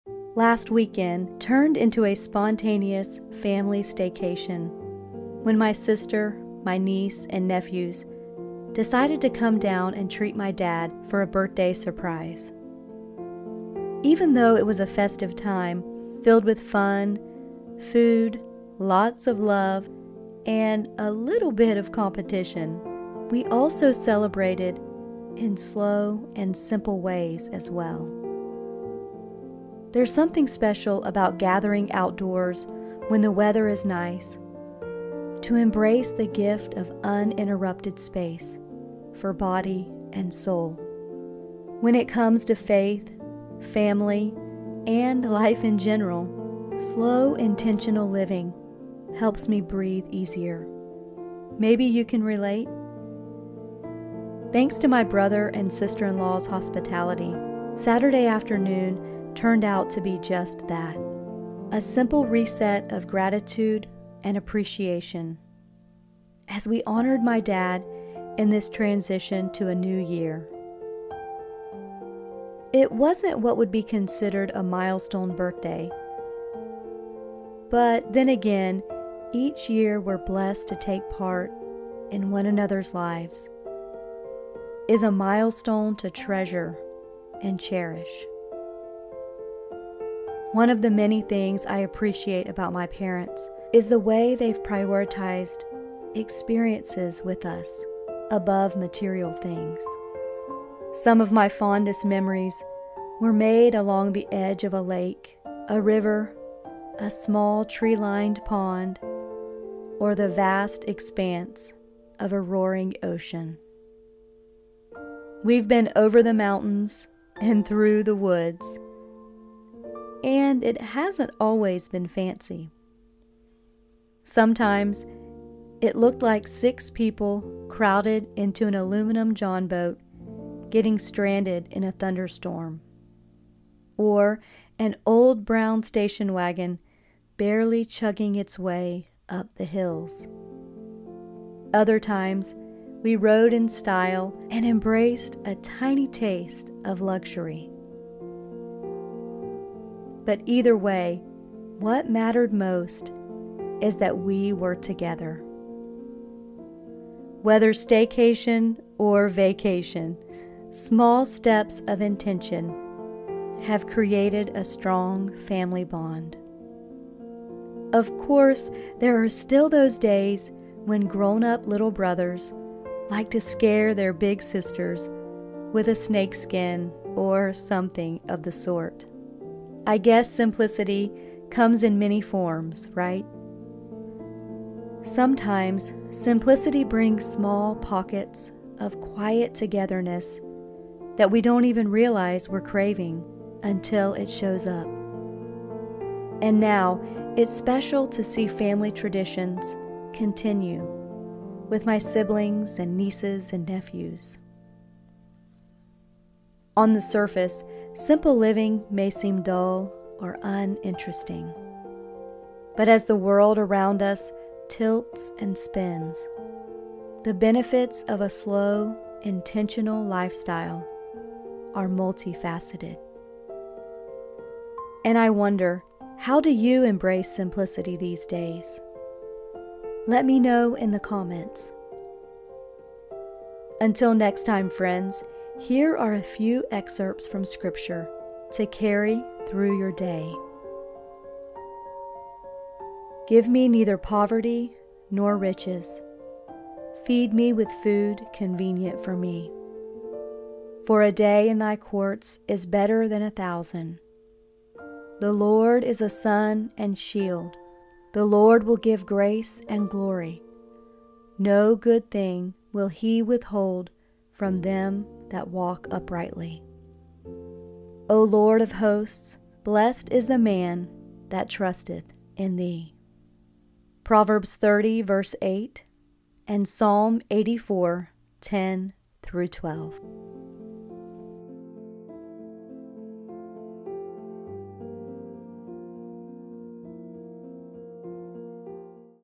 Fun Staycation + Simplicity for the soul
Last weekend turned into a spontaneous family staycation when my sister, my niece and nephews decided to come down and treat my dad to a birthday surprise.

Even though it was a festive time, filled with fun, food, lots of love and a little bit of competition, we also celebrated in slow and simple ways as well.

There's something special about gathering outdoors when the weather is nice to embrace the gift of uninterrupted space for body and soul.

When it comes to faith, family, and life in general, slow intentional living helps me breathe easier.

Maybe you can relate? Thanks to my brother and sister in law's hospitality, Saturday afternoon turned out to be just that. A simple reset of gratitude and appreciation as we honored my Dad in this transition to a new year.

It wasn't what would be considered a milestone birthday, but then again, each year we're blessed to take part in one another's lives is a milestone to treasure and cherish.

One of the many things I appreciate about my parents is the way they've prioritized experiences with us above material things.

Some of my fondest memories were made along the edge of a lake, a river, a small tree lined pond or the vast expanse of a roaring ocean.

We've been over the mountains and through the woods - and it hasn't always been fancy.

Sometimes it looked like six people crowded into an aluminum johnboat getting stranded in a thunderstorm, or an old brown station wagon barely chugging its way up the hills. Other times, we rode in style and embraced a tiny taste of luxury.

But either way, what mattered most is that we were together. Whether vacation or staycation, small steps of intention have created a strong family bond.

Of course, there are still those days when grown up little brothers like to scare their big sisters with a snake skin or something of the sort. I guess simplicity comes in many forms, right?

Sometimes, simplicity brings small pockets of quiet togetherness that we don't even realize we're craving until it shows up.

And now, it's special to see family traditions continue with my siblings, and nieces and nephews. On the surface, simple living may seem dull or uninteresting, but as the world around us tilts and spins, the benefits of a slow, intentional lifestyle are multifaceted. And I wonder, how do you embrace simplicity these days?

Until next time friends, here are a few excerpts from Scripture to carry through your day:

Give me neither poverty nor riches; feed me with food convenient for me.

For a day in thy courts is better than a thousand. The Lord is a sun and shield: the Lord will give grace and glory: no good thing will he withhold from them that walk uprightly. O Lord of hosts, blessed is the man that trusteth in thee.